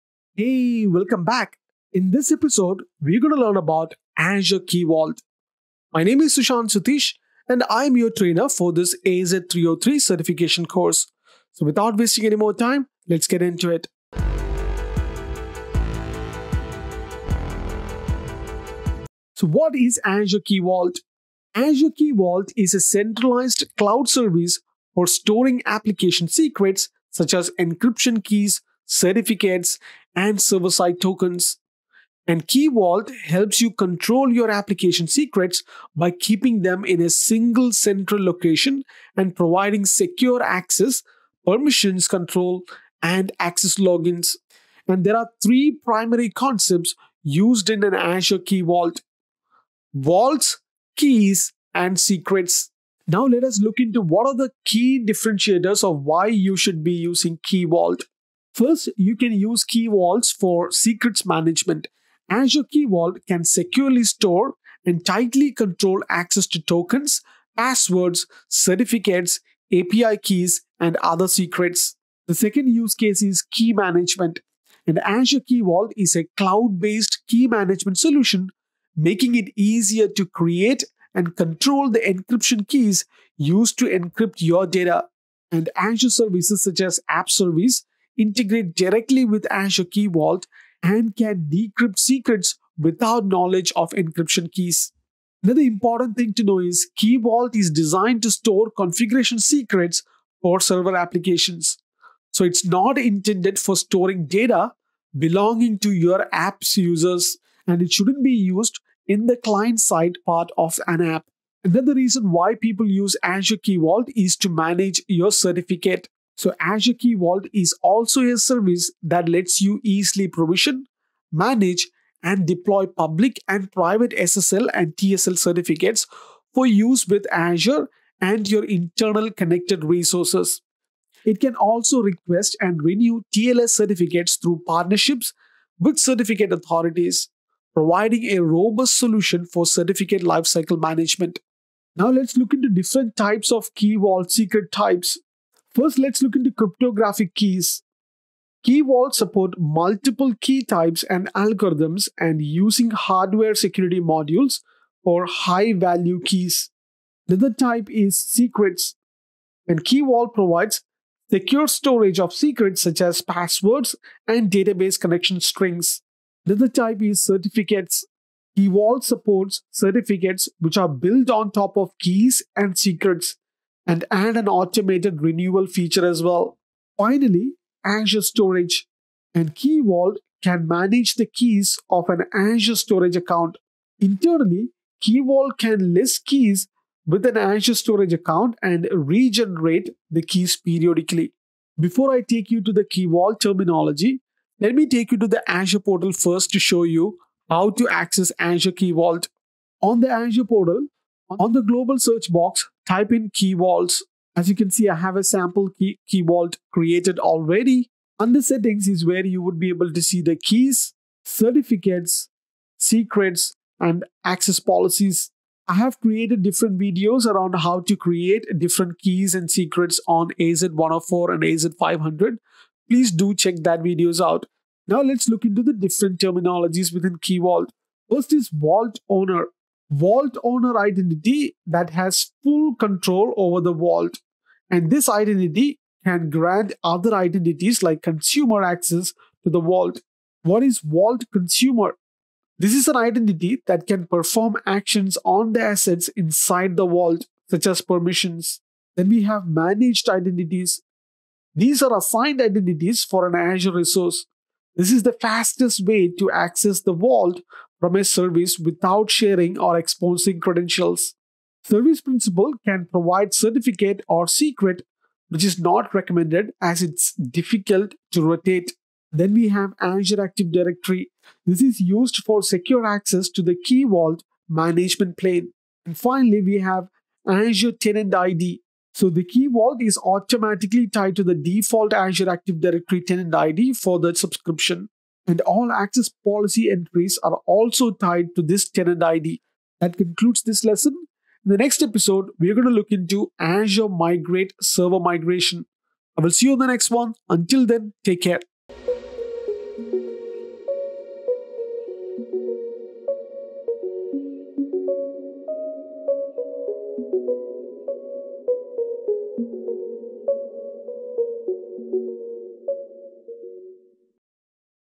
AZ-303 Exam EP 42: Azure Key Vault
⏰ Timestamps:

00:00​​​​​ ⏩ Introduction
00:19​​ ⏩ Overview Of Azure Key Vault
00:53​​​​​ ⏩ Key Vault Uses
02:22​​​​​ ⏩ Key Vault Secret Types
03:12​​​​ ⏩ Key Vault Terminology
04:50​​​​​ ⏩ What's Next